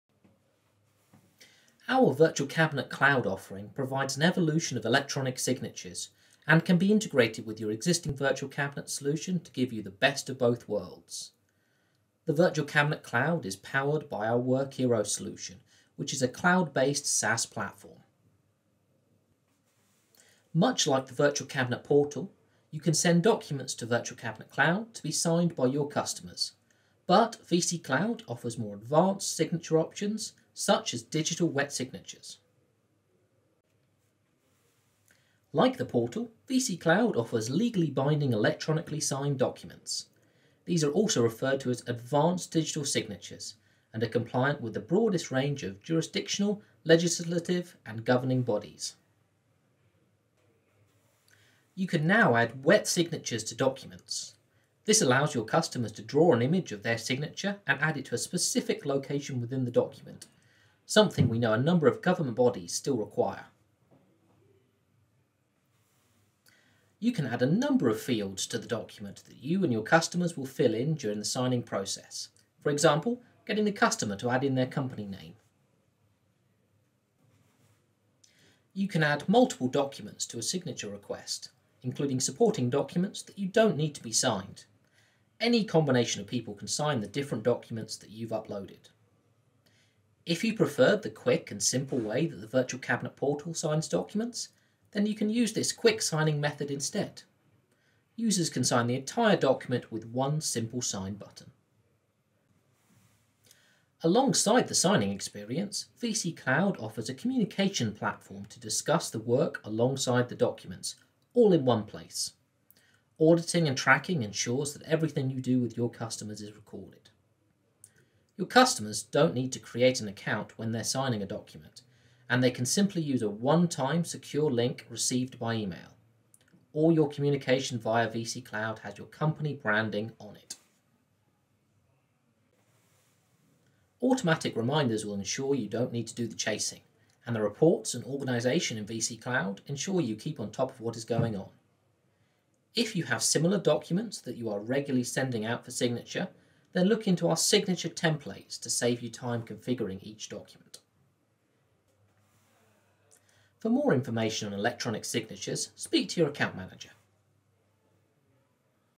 Digital Wet Signatures | Virtual Cabinet
Our wet digital signatures give senders and recipients the flexibility to choose how to sign a document. Sign directly using a mouse or a trackpad, or type using your keyboard. If a simple agreement is required then our one-click signing option is perfect.
Find out more in this quick video.